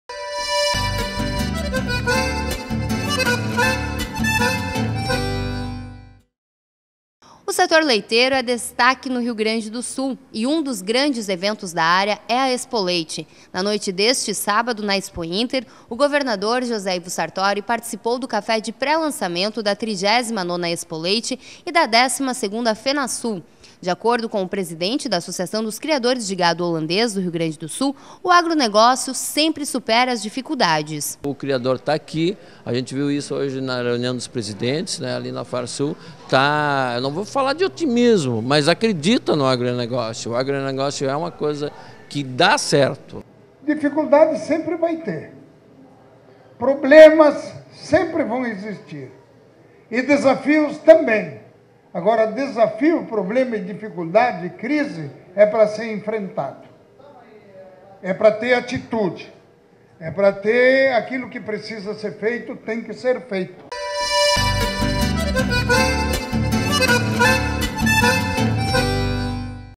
Pré-lançamento da Expoleite 2016 é realizado na Expointer
A Associação dos Criadores de Gado Holandês do Rio Grande do Sul (Gadolando) fez, na noite desse sábado, o pré-lançamento da 39º Expoleite e 12º Fenasul, que acontecerá no período de 18 a 22 de maio de 2016, no Parque Assis Brasil, em Esteio. A cerimônia foi realizada na Casa da Gadolando, durante a 38a Expointer. O ato contou com as presenças do governador José Ivo Sartori e do presidente da entidade, Marcos Tang.